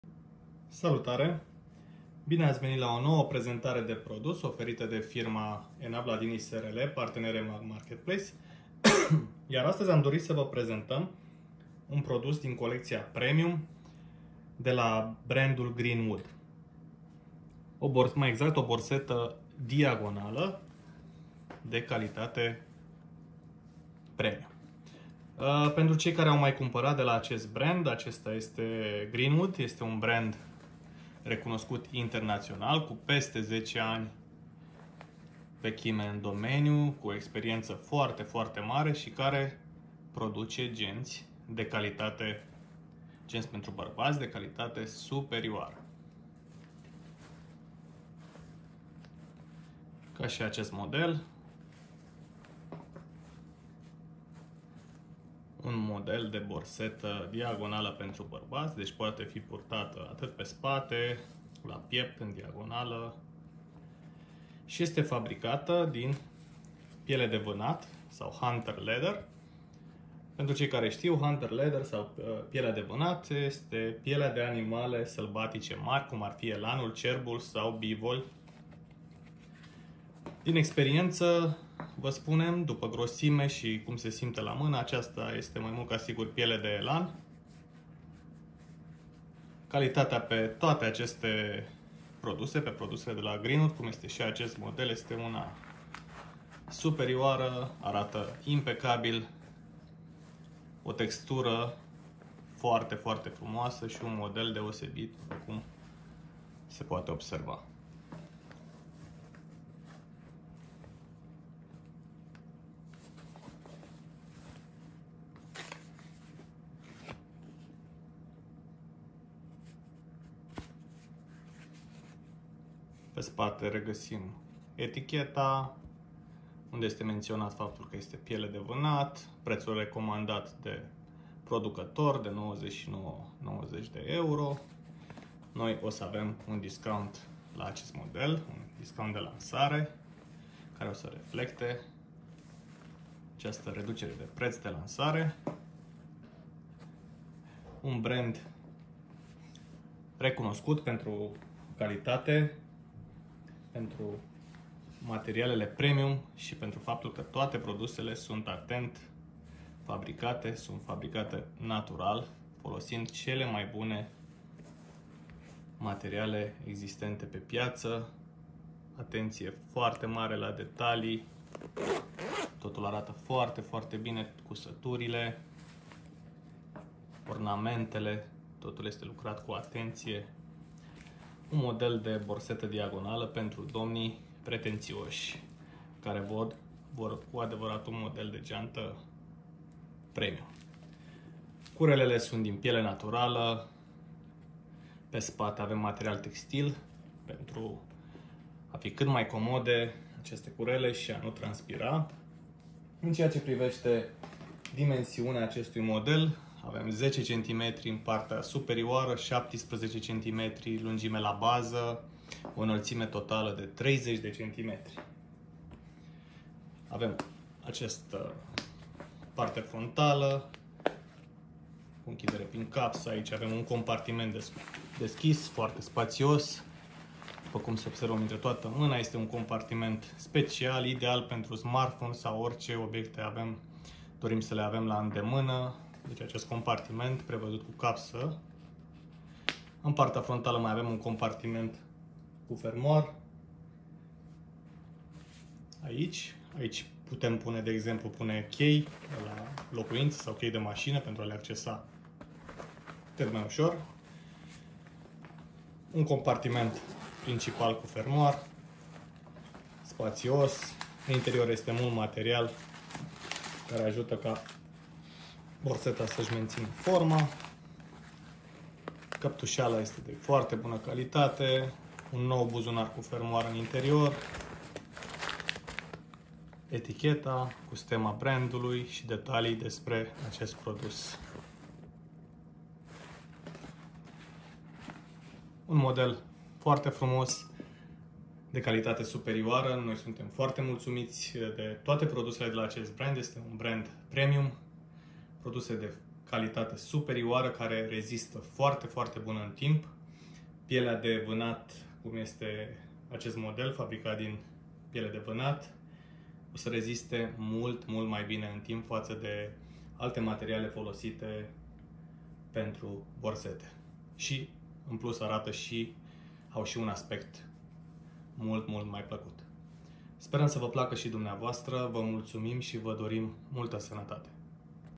Borseta Barbati Piept, Crossbody din Piele Naturala, Green WOOD, Premium, Hand Made, Maro Camel
Cumpara | Borseta/Geanta Barbati Green WOOD din Piele naturala, Mod Purtare: La Piept, Crossbody, Pe Umar. Confectionata manual din piele naturala de bovina aceasta borseta de calitate premium ofera spatiu suficient pt depozitare intr-un design compact.

Aceasta borseta este mult mai mult decat o simpla geanta; este un partener de incredere in fiecare aventura pe care o intalniti. Cu materiale de cea mai buna calitate, un design sofisticat si compartimentare bine gandita, Green Wood va aduce o geanta crossbody care nu numai ca indeplineste cerintele dumneavoastra practice, dar si va reflecta stilul si gustul in modul cel mai elegant posibil.

Asadar, insotiti-ne in aceasta prezentare detaliata a Borsetei crossbody pentru barbati Green Wood, si descoperiti de ce este atat de speciala, de ce atrage atentia si cum va poate imbunatati modul de a va purta obiectele personale intr-un mod cu adevarat elegant si rafinat.

Compartimentare si Caracteristici
Dimensiuni: 10 cm lungime in partea superioara si 17 cm lungime la baza, 30 cm inaltime, 10 cm grosime
Bretele din piele naturala captusite cu material textil pe partea inferioara pentru confort maxim
Sigla Greenwood în relief pe exterior, etichetă cusută în compartimentul principal cu sigla si detaliile brandului: Green Wood
Parte dorsala din material textil respirabil - captusit cu spuma EVA in interior pentru extra confort
Compartiment principal de depozitare prevazut cu fermoar
Compartiment cu fermoar in partea frontala: 12 x 13 cm - ideal pt chei
Compartiment frontal cu capsa 16 x 16 cm
Compartiment dorsal cu fermoar 13 x 16 cm